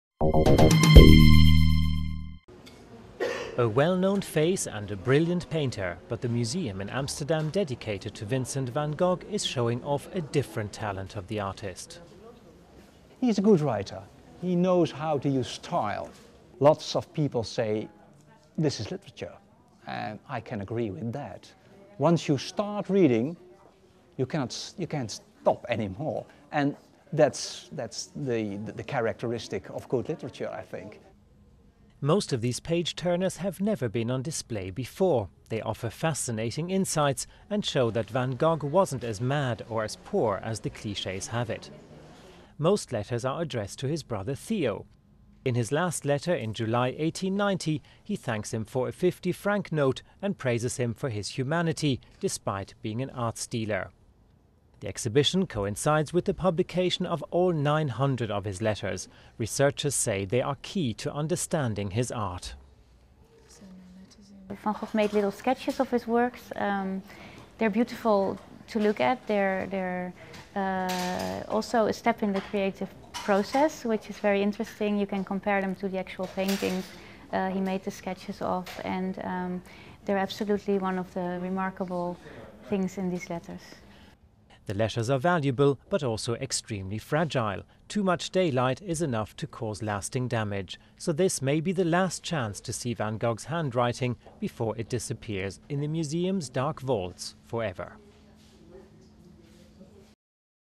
Van Gogh letters on rare display in Amsterdam
The letters of Vincent Van Gogh are a famous record of the creative genius of the painter. Now the Van Gogh museum in Amsterdam is putting 120 of them on show in an extremely rare exhibition called "The Artist Speaks". Duration : 01:41